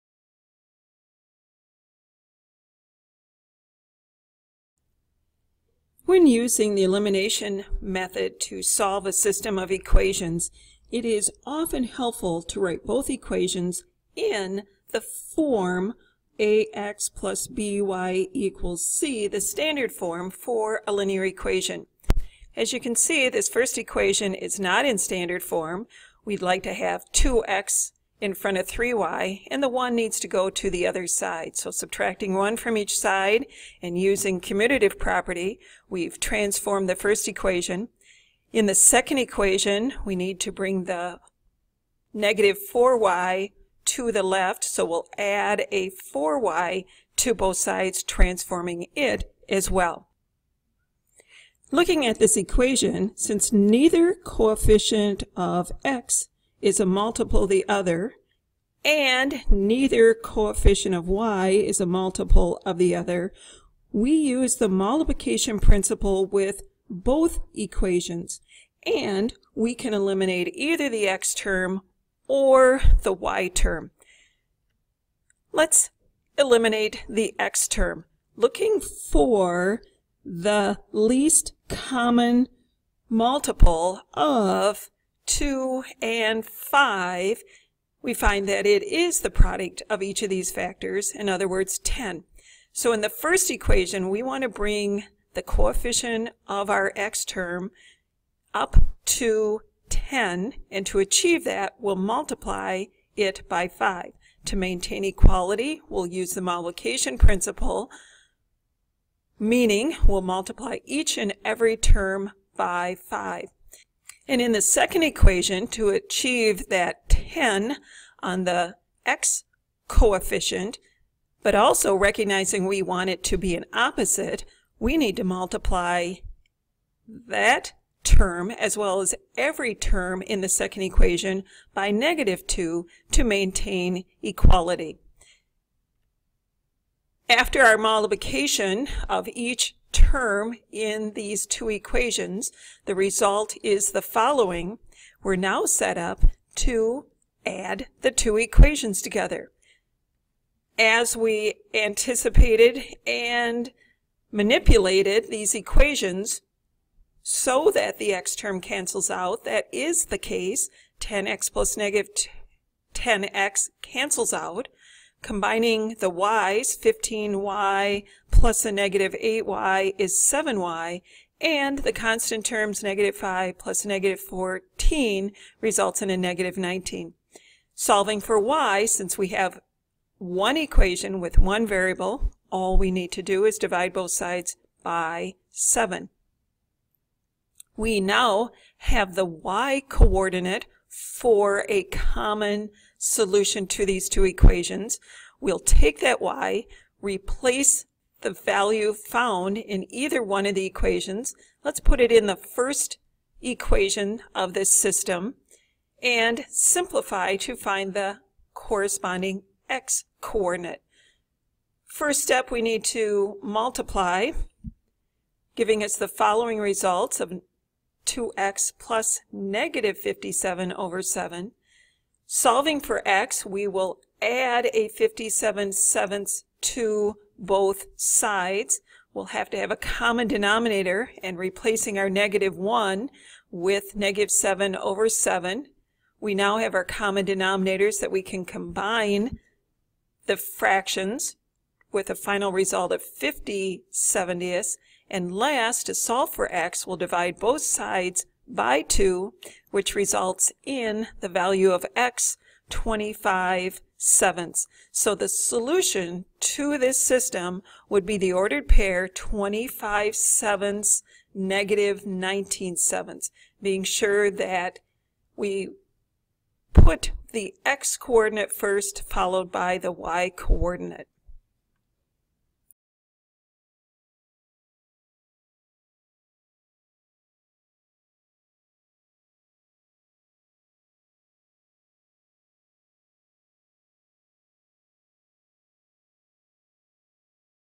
Elimination Method when Both Equations Need Multiplication - Elementary Algebra
Learn how to apply multiplication to both of the given equations in a system in order to use the elimination method for solving a system of equations.

This lecture video for Elementary Algebra is presented by Southwest Wisconsin Technical College mathematics instructor Helen Mar Adams.